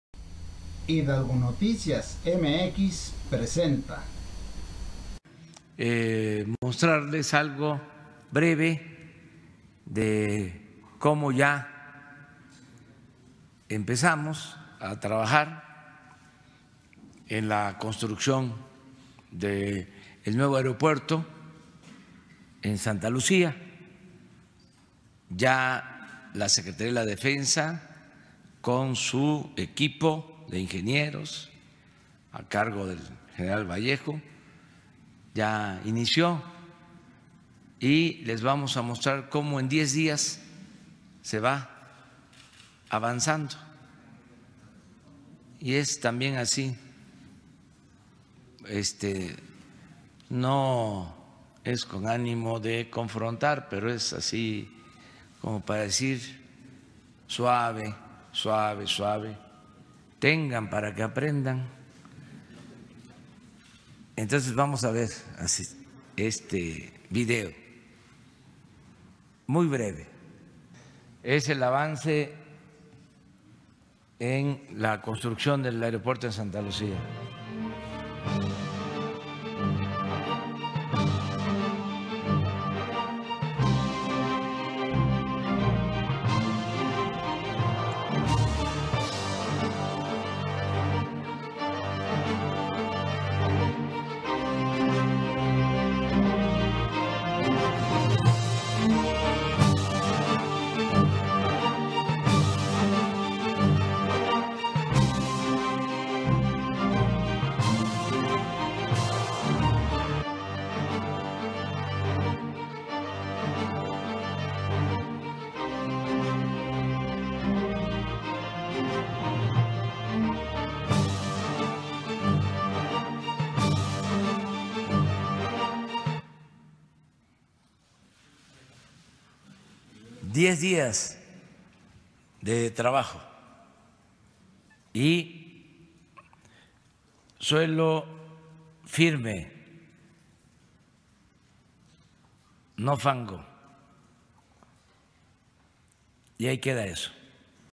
AMLO: Tengan para que aprendan. Suelo firme, no fango. Comenzamos a trabajar en el NA de Santa Lucía
AMLO mostró en la mañanera un video de los avances que llevan en 10 días de construcción del nuevo aeropuerto de Santa Lucía.